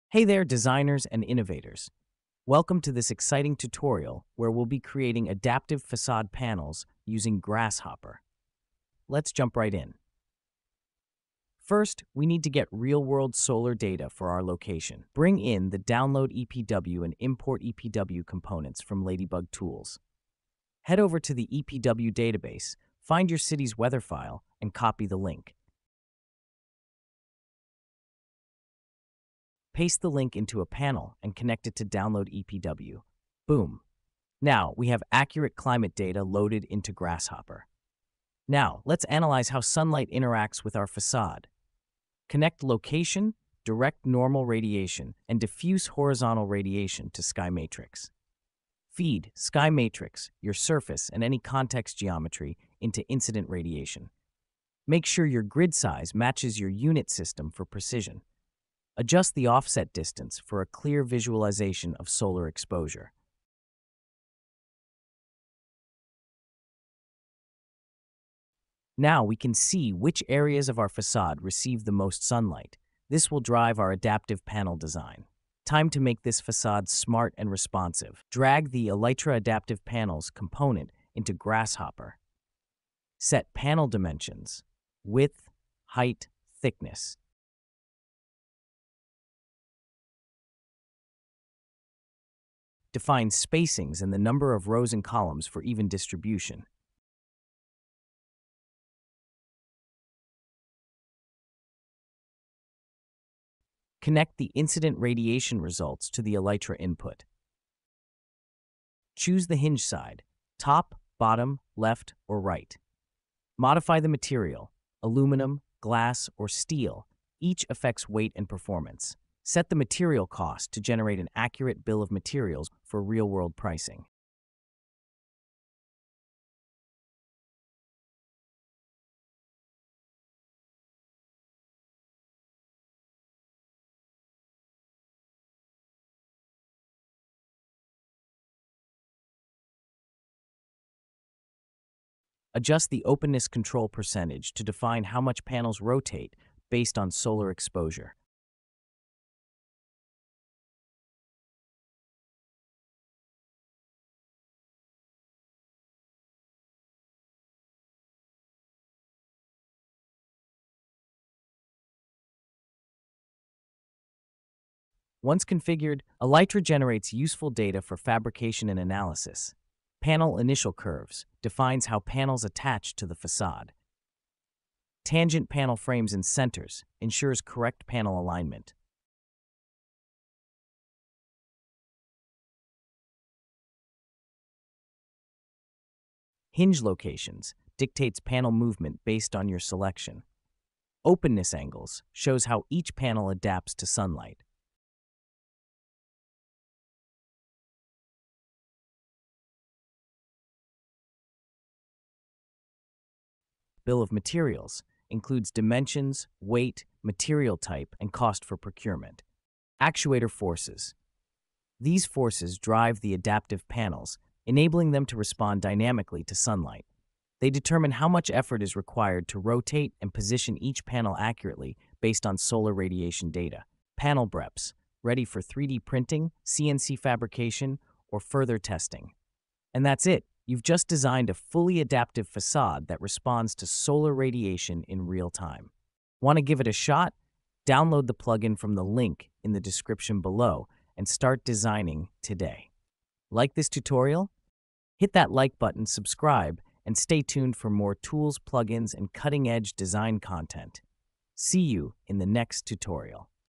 Adaptive Facade Panels in Grasshopper – Parametric Design for Dynamic Architecture!
🚀 Transform your facades with adaptive, parametric panels in Grasshopper!

In this video, we introduce Adaptive Facade Panels (Elytra AdPanels), a Grasshopper plugin that allows you to create intelligent, responsive facades that adapt based on solar radiation and spacing control.